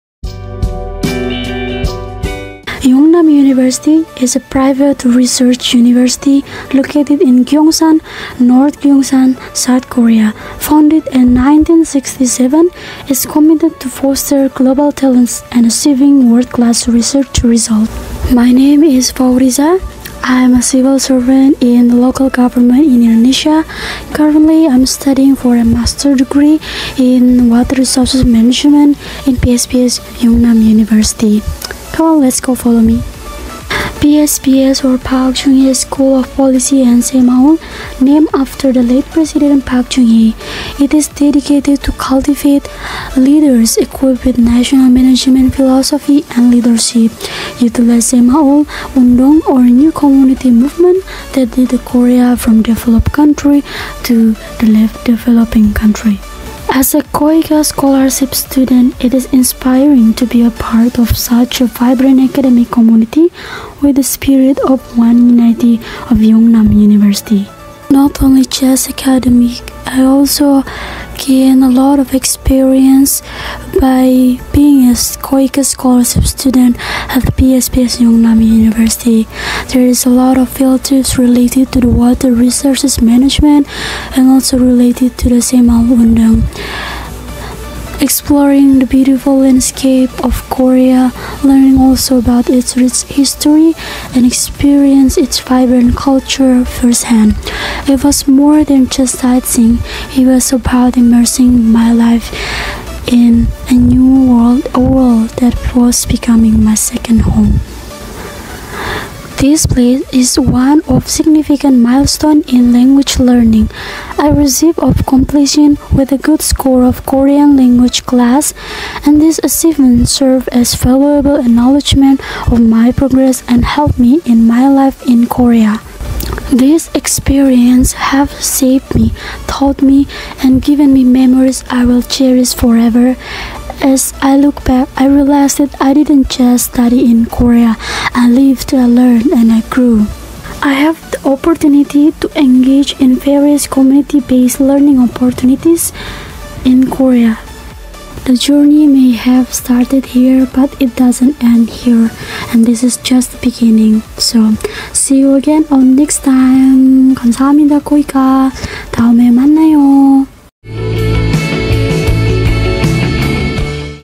2024 KOICA SP_Yeungnam University_Patirajawane Fauriza
University
- Yeungnam University

Course Title
- The Master’s Degree Program in Water Resources Management

Title of Film
- "Lighting the Path to Excellence as KOICA Scholarship Student of Water Resources Management Major in PSPS, Yeungnam University”

Description of Film
- My name is Fauriza Patirajawane, I am working as East Sumba Regency Local Government officer back in Indonesia. Currently I am studying Master’s Degree since August 2023 with scholarship from KOICA Scholarship Program (KOICA：SP) Student with major of Water Resources Management Major in Park Chung Hee School of Policy and Saemaul (PSPS), Yeungnain University, located in Gyeongsan-si, South Korea. This video provides an in-depth look into my lite as KOICA SP student in South Korea and academic life at Yeungnam University. We are the first batch of Water Resources Management Program in Yeungnam University with KOICA SP. In this video, I will share my experiences, showcase my beautiful campus and the opportunities provided by KOICA Scholarship. This video is being taken for 2024 KOICA Youtube Contest for SP Fellows.
Leave your questions and comments below, and we’ll do our best to address them in liiture videos.